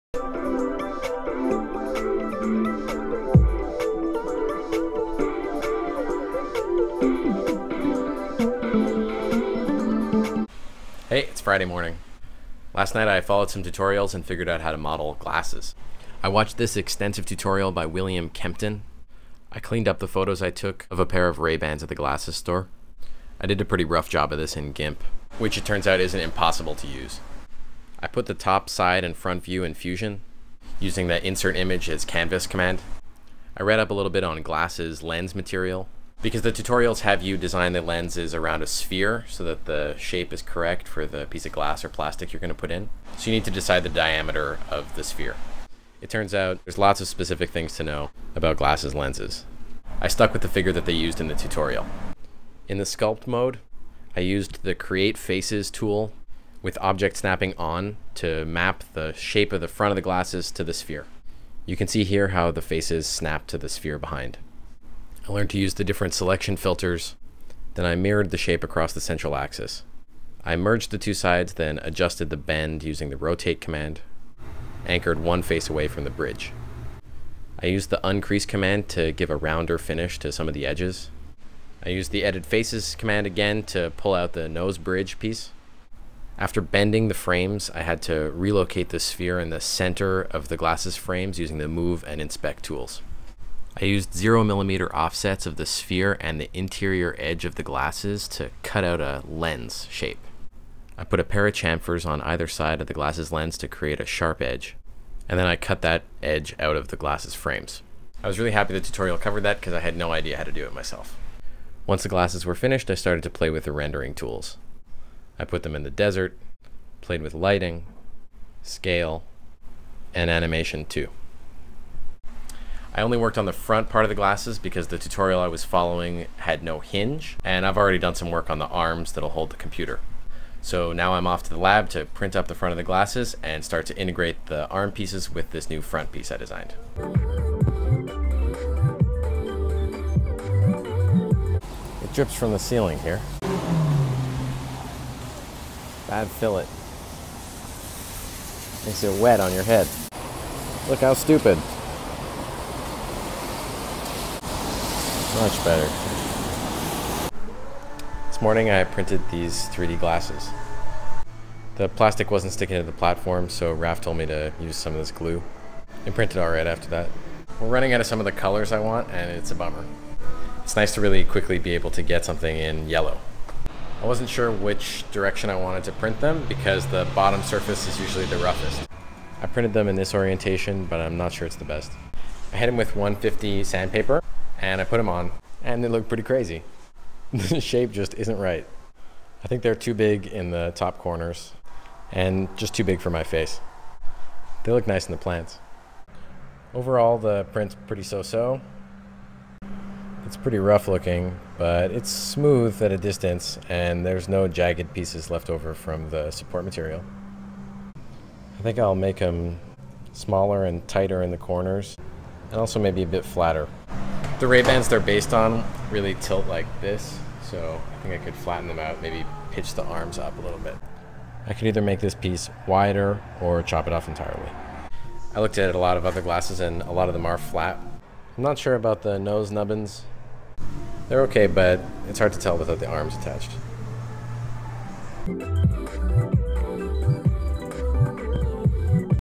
Designing and Printing Glasses in Fusion360 - Field Notes 17
in this episode:
- i design my first pair of glasses frames with Fusion 360's sculpt tool
- i print them on the 3d printer
- i review their shortcomings